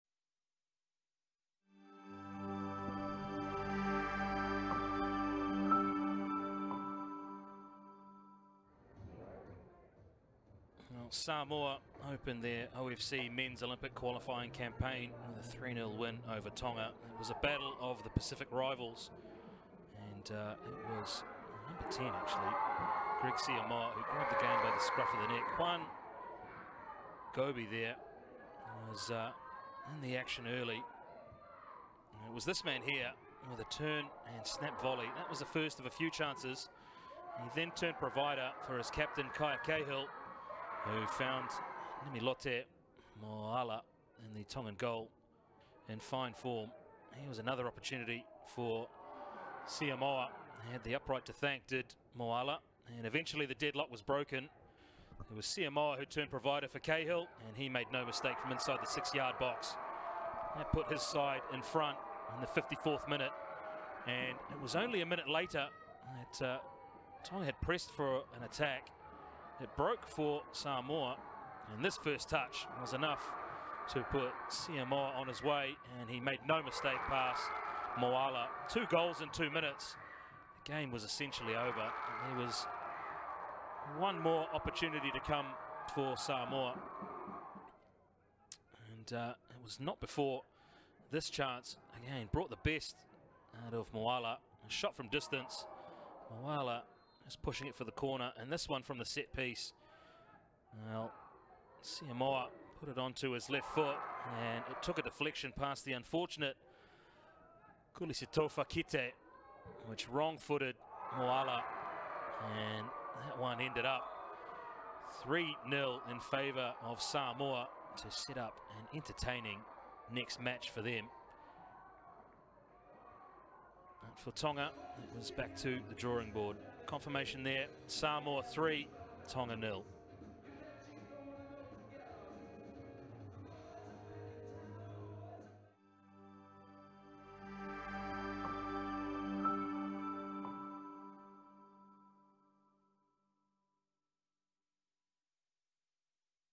OFC Men's Olympic Qualifier 2023 | Samoa v Tonga | Highlights
Samoa and Tonga meet in the opening match in Group B at the OFC Men's Olympic Qualifier in Auckland.